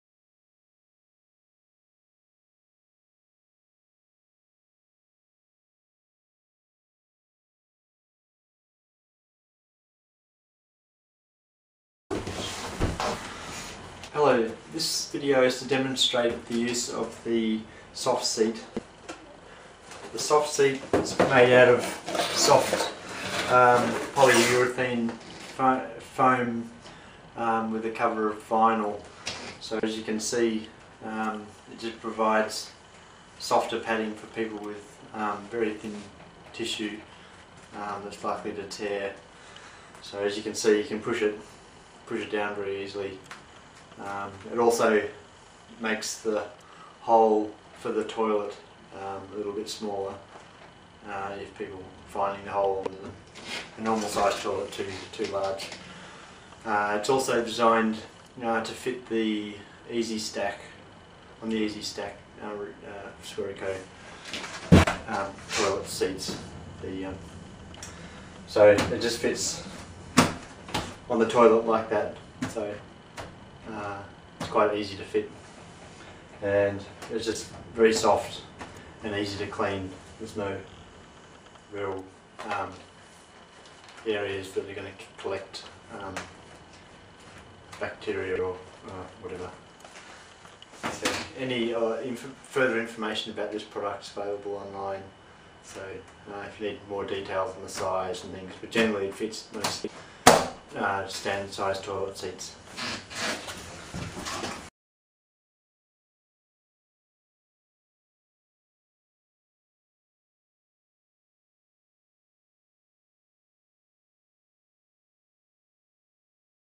Softseat REKO
A soft toilet seat for people who needs a soft seat and/or a smaller hole. Made of polyurethane foam with a layer of vinyl. Especially made for Swereco Stacking Toilet Seat but fits most of the seats in the market.